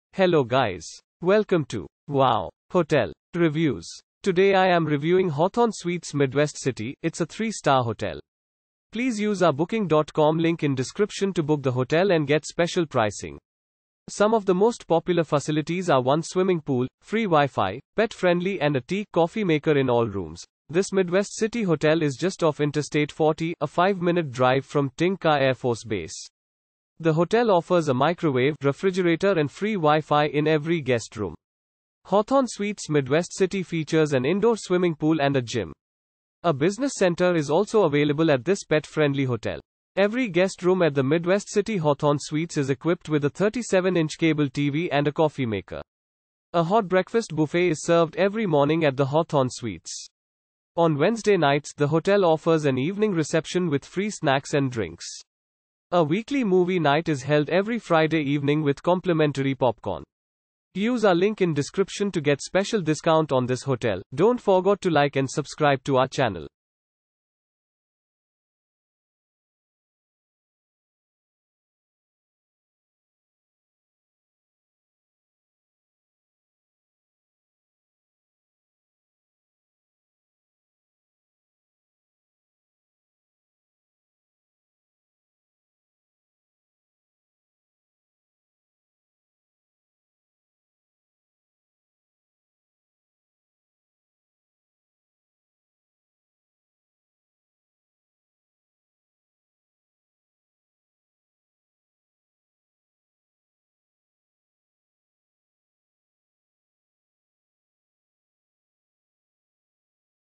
Hawthorn Suites Midwest City Review - Midwest City , United States of America
Hawthorn Suites Midwest City

This Midwest City hotel is just off Interstate 40, a 5-minute drive from Tinker Air Force Base. The hotel offers a microwave, refrigerator and free Wi-Fi in every guest room.
Hawthorn Suites Midwest City features an indoor swimming pool and a gym. A business center is also available at this pet-friendly hotel.
Every guest room at the Midwest City Hawthorn Suites is equipped with a 37-inch cable TV and a coffee maker.
A hot breakfast buffet is served every morning at the Hawthorn Suites. On Wednesday nights, the hotel offers an evening reception with free snacks and drinks. A weekly movie night is held every Friday evening with complimentary popcorn.